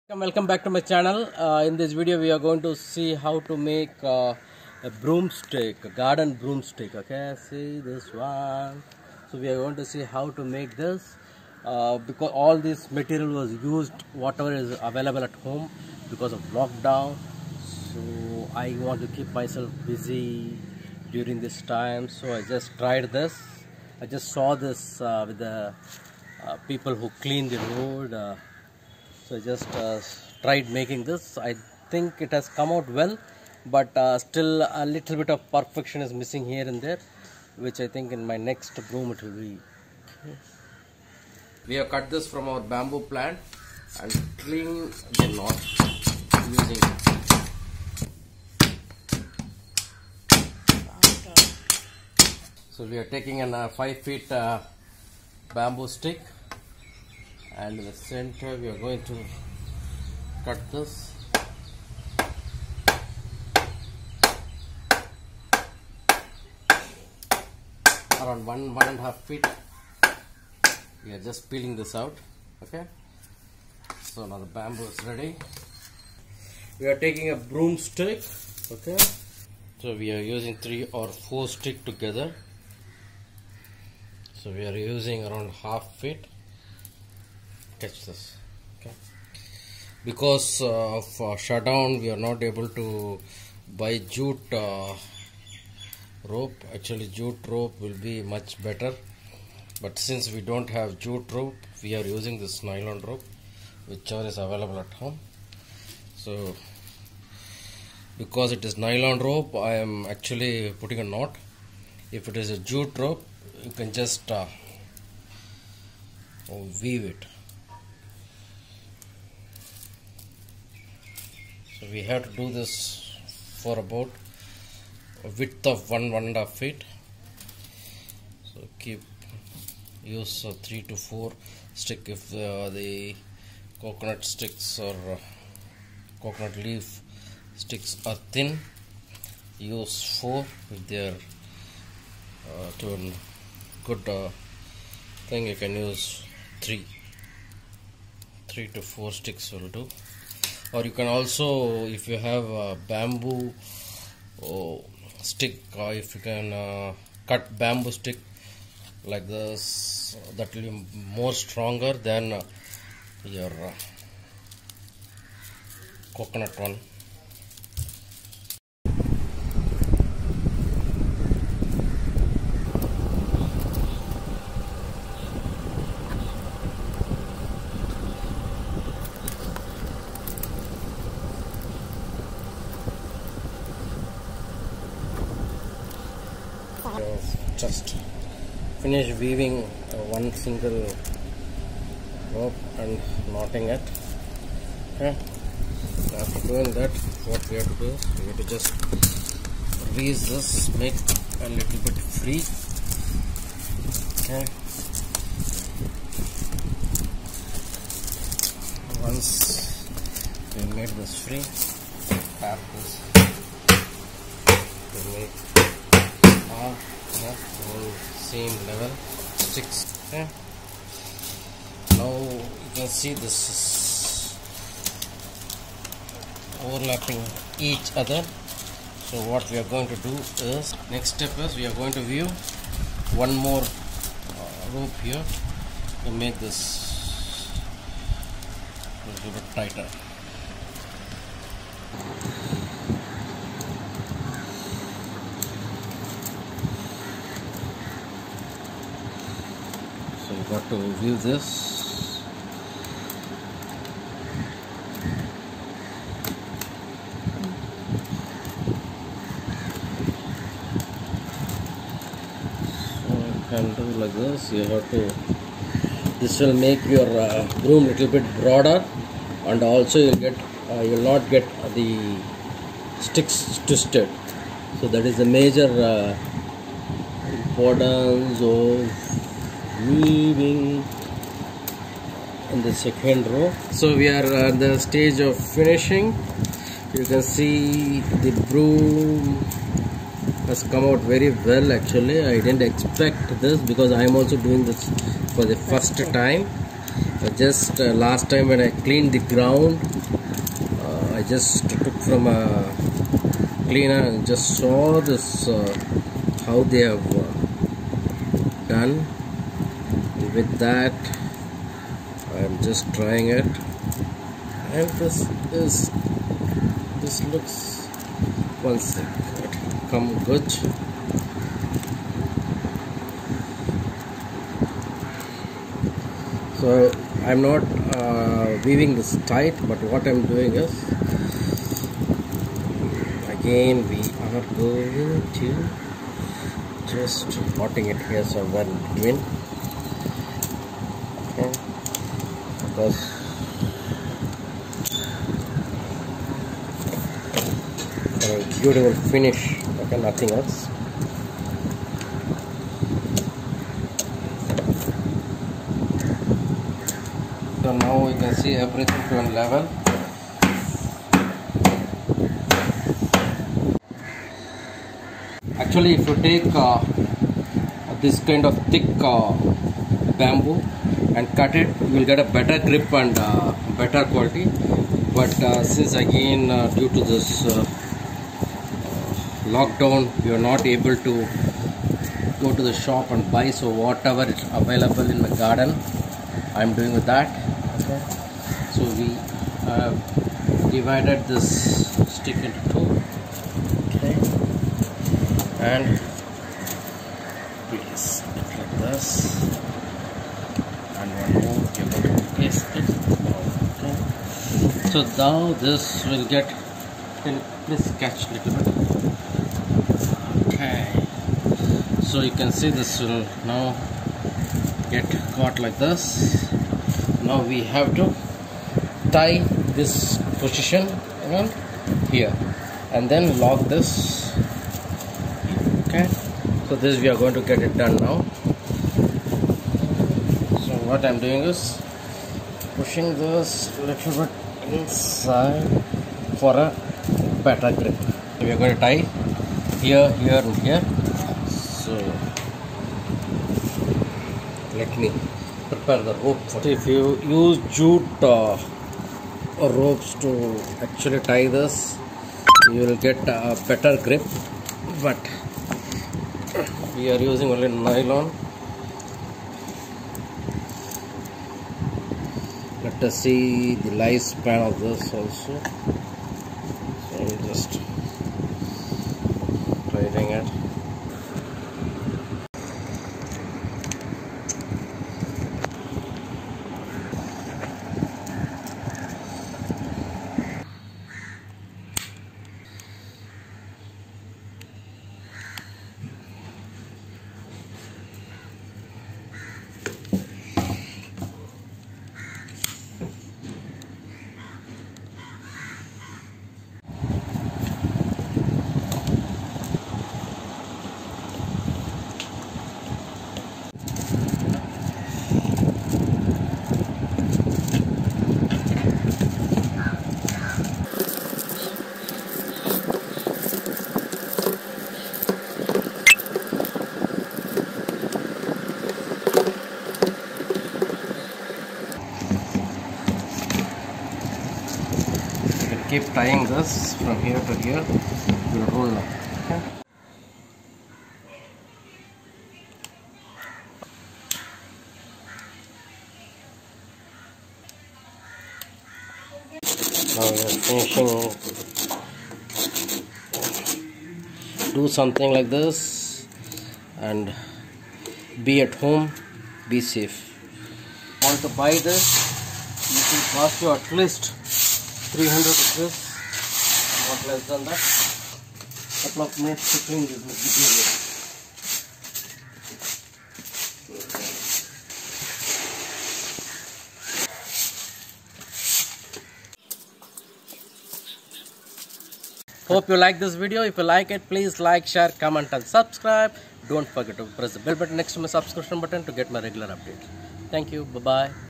DIY | GARDEN BROOM | KRISHNA KAANTH
In this video we are going to see how to make garden broom stick using the materials available at home due to lock down.